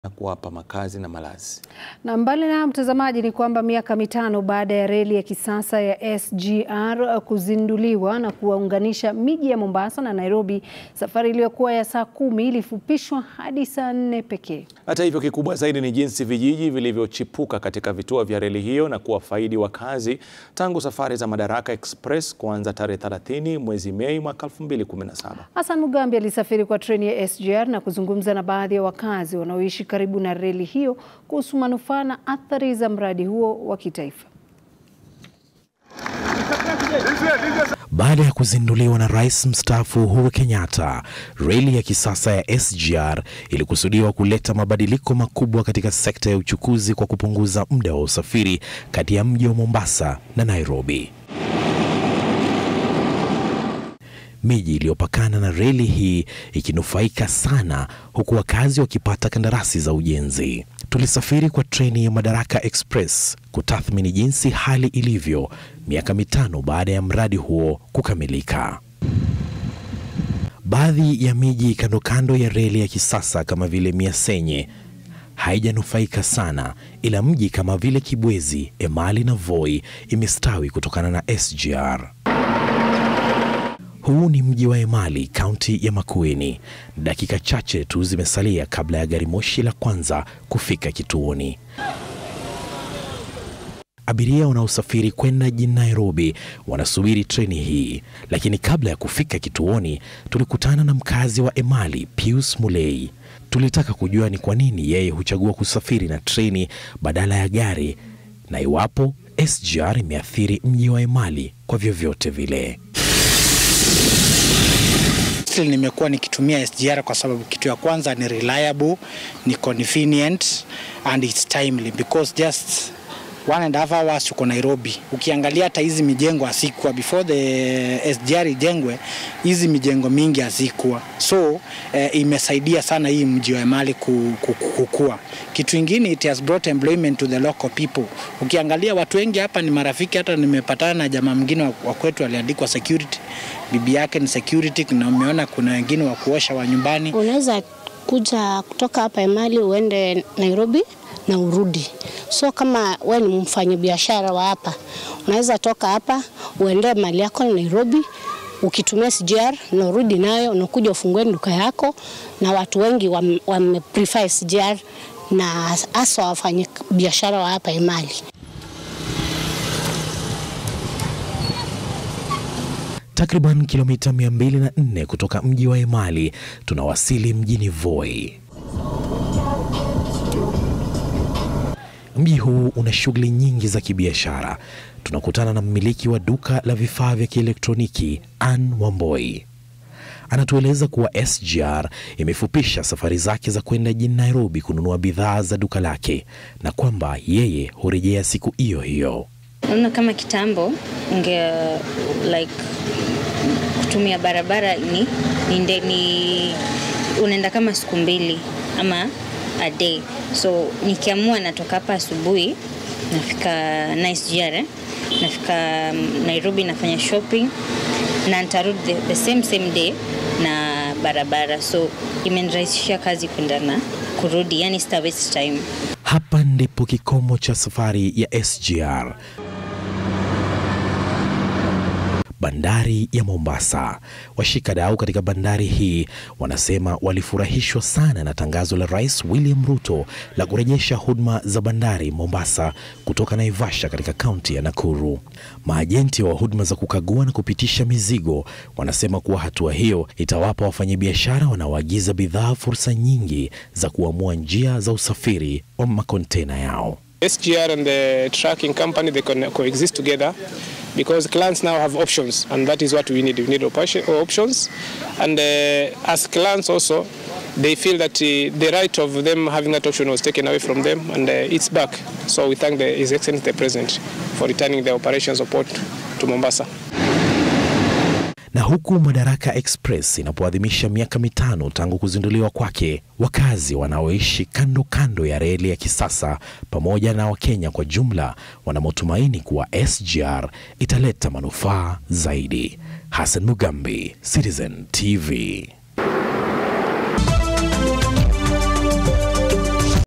Safari ya SGR | Reli ya kisasa ilikusudiwa kuleta mabadiliko makubwa nchini
Miaka mitano baada ya reli ya kisasa SGR kuzinduliwa na kuunganisha miji ya Mombasa na Nairobi, safari iliyokuwa ya saa 10 ilifupishwa hadi saa nne pekee. Hata hivyo, kikubwa zaidi ni jinsi vijiji vilivyochipuka katika vituo vya reli hiyo na kuwafaidi wakazi tangu safari za Madaraka Express kuanza tarehe 31 mwezi Mei, 2017.